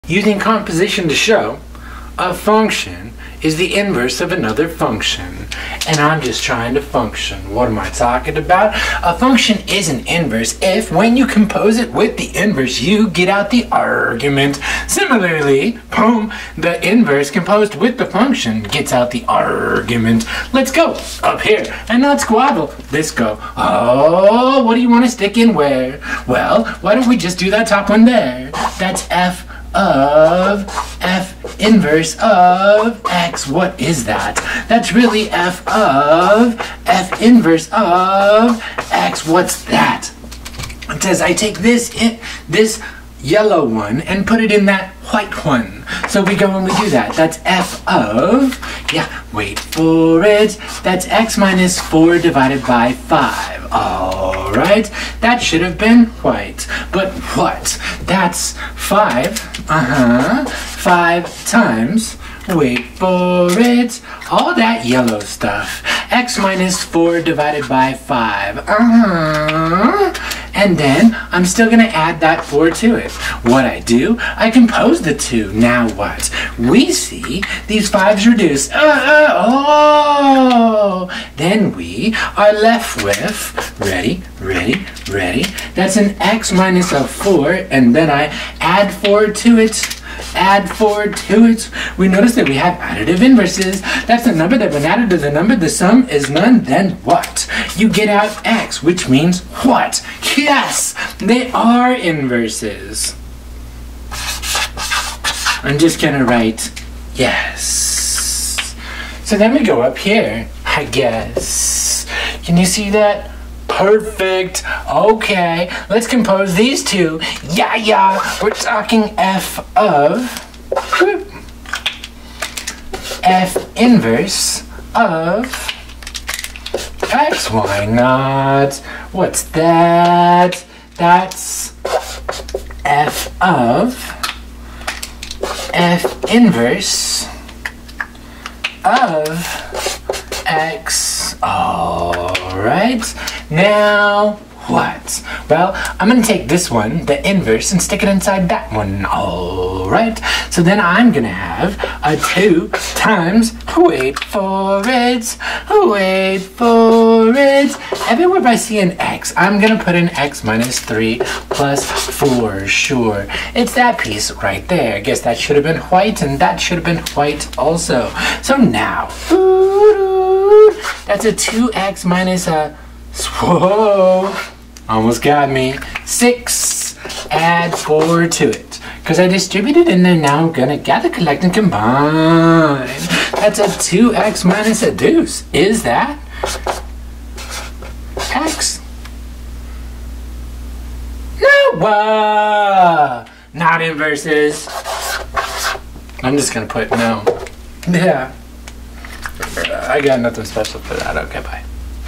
Check function inverses using composition
Use composition to check for inverse functions. FoF-1x=x. This video will show you two examples of using composition to determine if a function is an inverse of another function. the first example is and inverse the second is not.